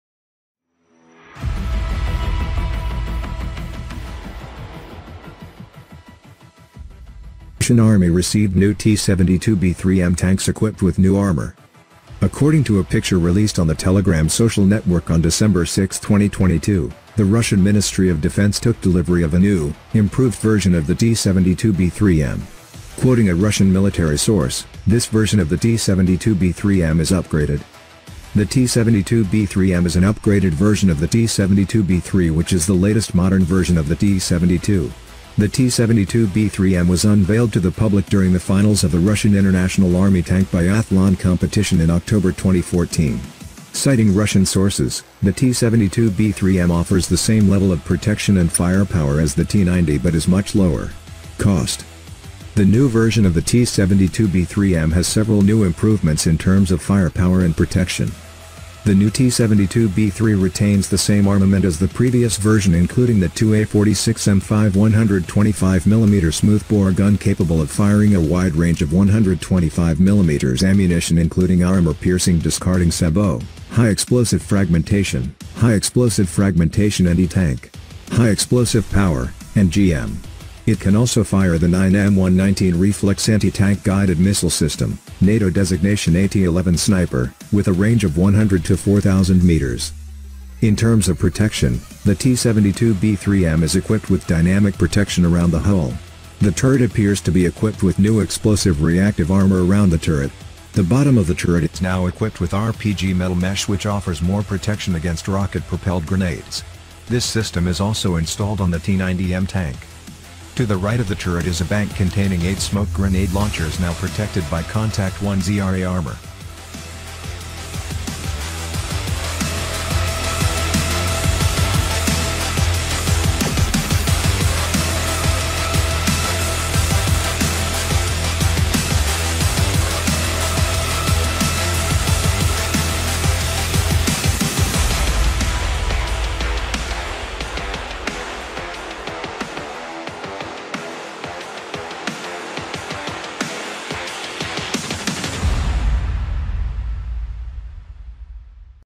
The Russian army received new T-72B3M tanks equipped with new armor
The T-72B3M is an upgraded version of the T-72B3 which is the latest modern version of the T-72. The T-72B3M was introduced to the public during the Finals of the Russian International Army Tank Biathlon Competition in October 2014. Citing Russian sources, the T-72B3M offers the same level of protection and firepower as the T-90 but is much lower. cost.
The new version of the T-72B3M has several new improvements in terms of firepower and protection.
In terms of protection, the T-72B3M is equipped with dynamic protection around the hull. The turret appears to be equipped with new Explosive Reactive Armor around the turret. The bottom of the turret is now equipped with RPG metal mesh which offers more protection against Rocket-Propelled Grenades. This system is also installed on the T-90M tank.